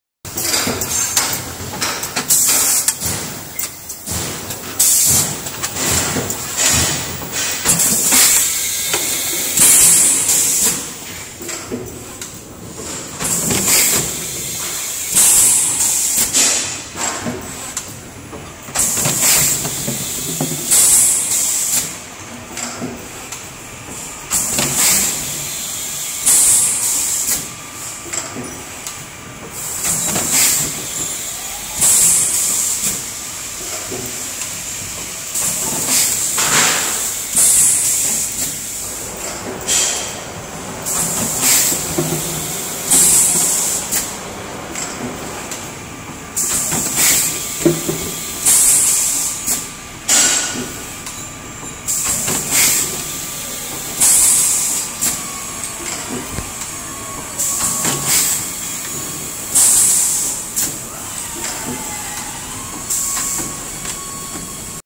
10L Full servo drivE FOR OIL BOTTLE BLOWING MACHINE
1.Make bottles for water, juice, liquor, syrups, jams, cooking oil, carbonated drinks, medicines, cosmetics, detergents and cleaning liquids, pesticides, candy jars, etc.
2.Downward servo stretching like all rotary machines.
3.Designed for no neck damage
4.Low noise level
5.Mould loading crane not required
6.Fast mould change time (15 min.)
7.Powerful oven to give very good bottle quality and quick start-up
8.Direct feed to air conveyor. No cumbersome bottle tilting or re-orientation required
9.Spindles designed to take any preform inner diameter variation
10.Highly productive, rugged machines for 24x7, 365 days working

PET 10L 2-Cavities Full-Auto full-Servo Blow Molding Machine,Bottle Making Machine,Bottle Blowing Machine  The company adopts Germany advanced technology to develop the BG Servo series PET high speed energy-saving full automatic bottle blowing machine with servo system for driving.